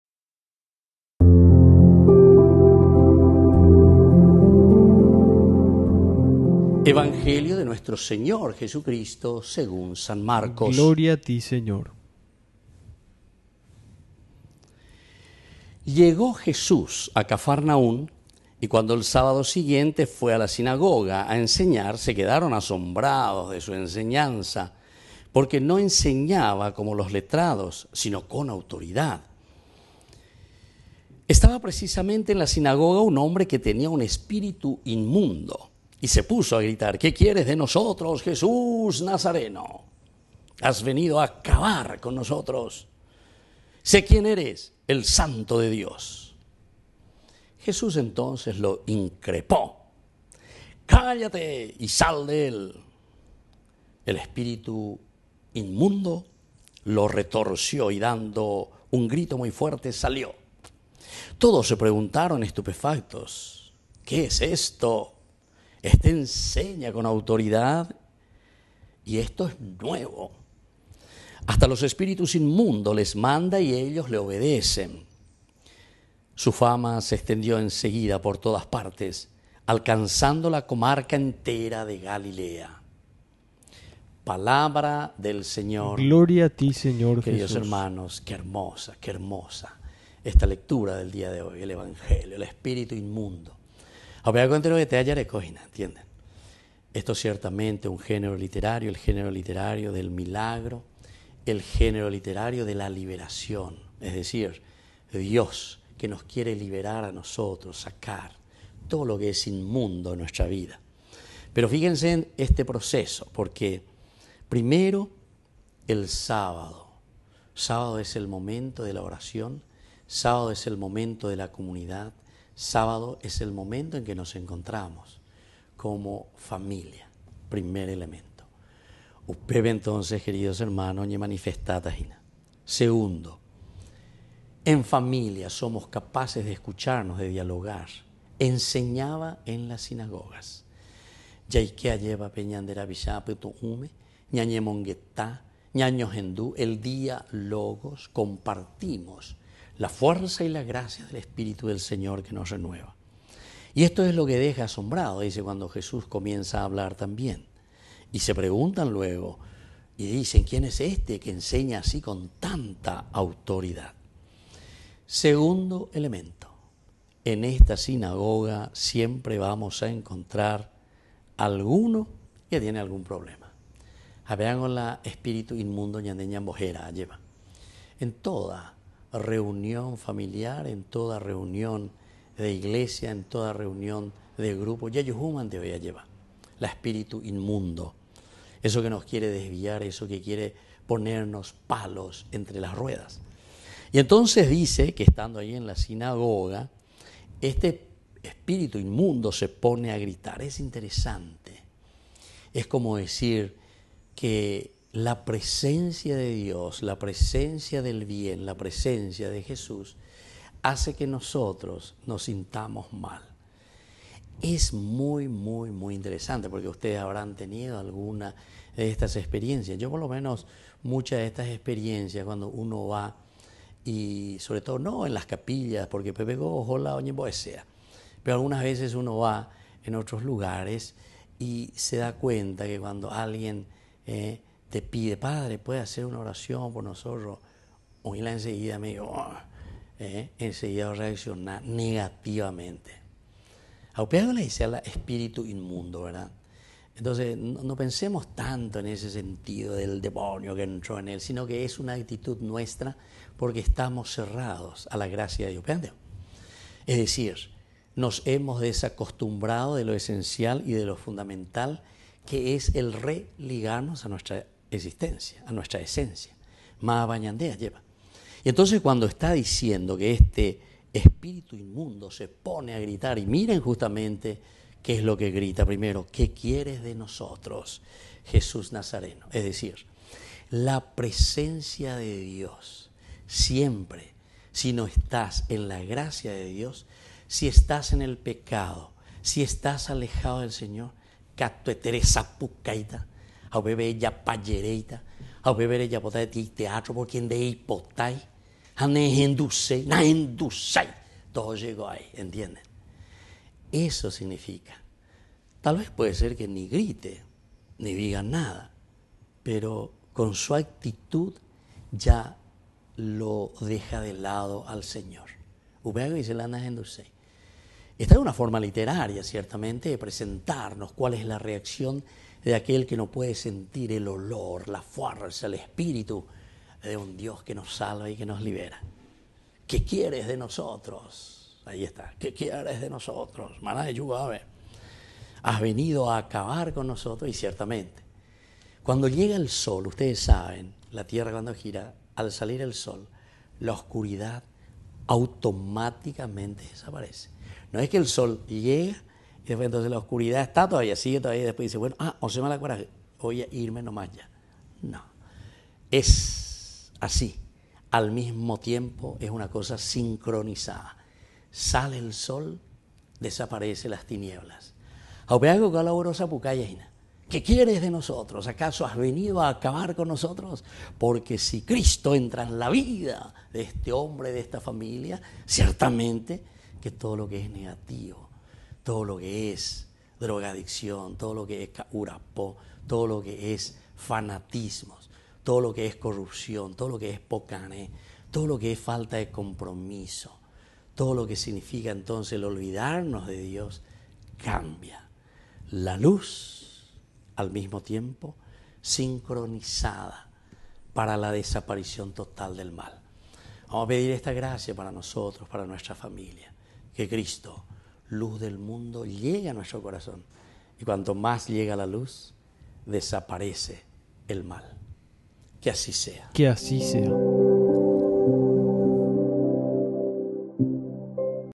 HOMILIA DE HOY | Martes 9 de enero del 2024.
Santo Evangelio según San Marcos 1, 21-28:
Sé quién eres, el Santo de Dios. Pero Jesús le reprendió, diciendo: ¡Cállate, y sal de él! Y el espíritu inmundo, sacudiéndole con violencia, y clamando a gran voz, salió de él.